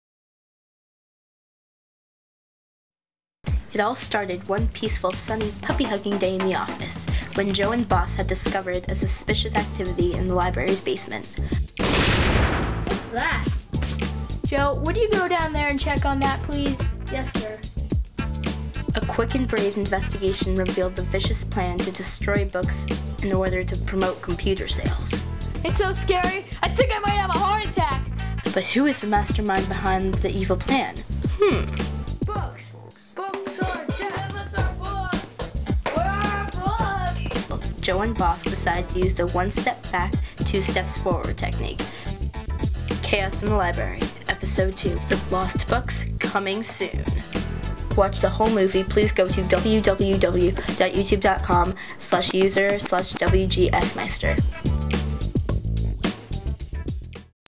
Chaos In The Library: Episode II - The Lost Books Trailer
This trailer is for the stop animation movie -  Chaos In The Library.
Fun to watch.